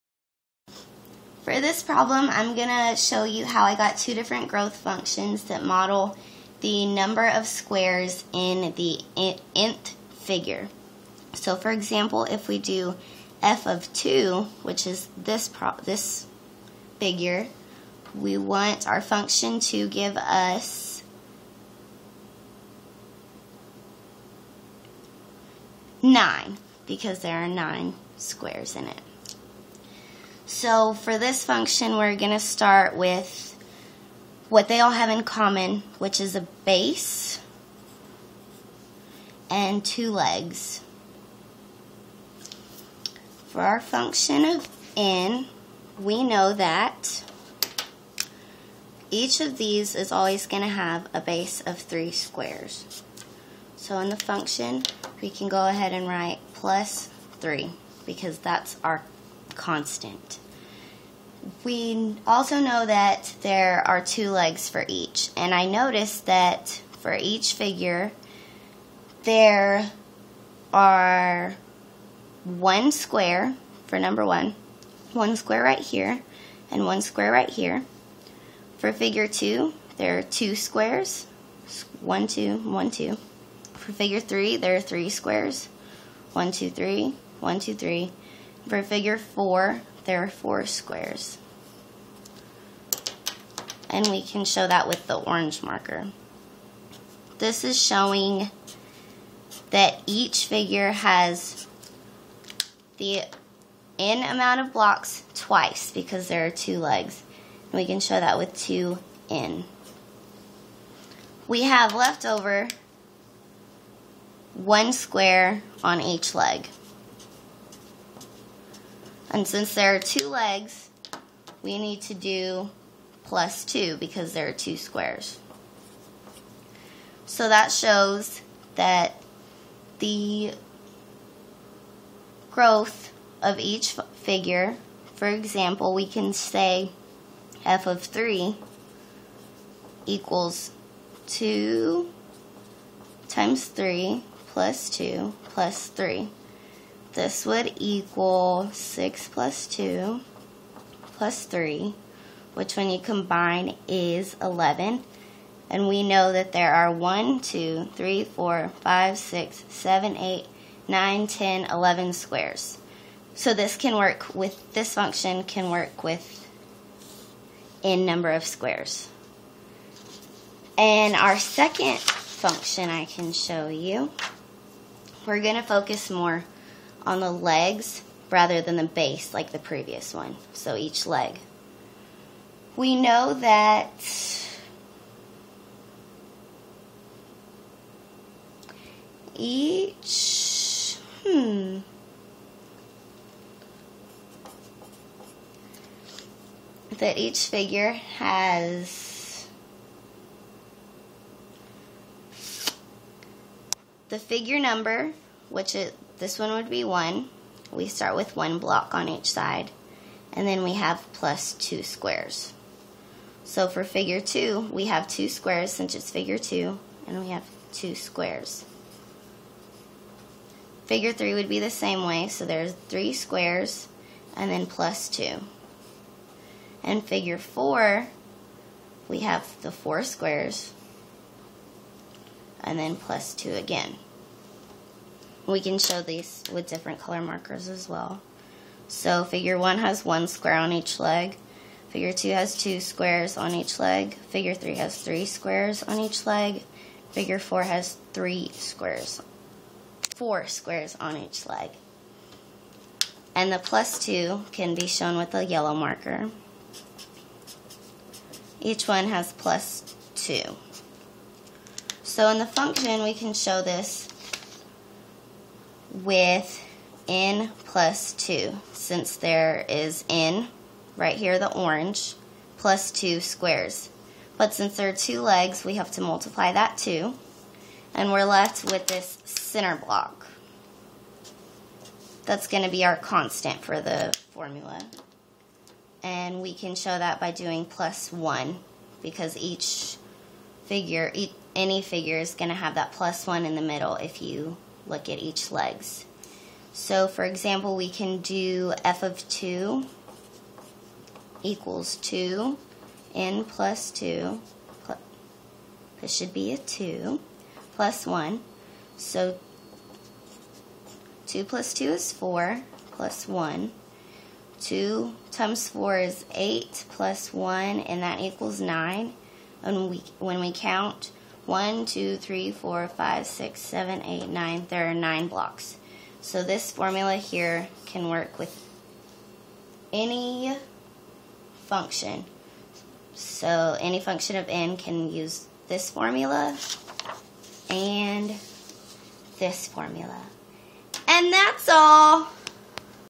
Growth Functions
Provide two different growth functions that model the figures below and describe how each function relates to the figures geometrically. (The function should give us the number of squares in the nth figure.)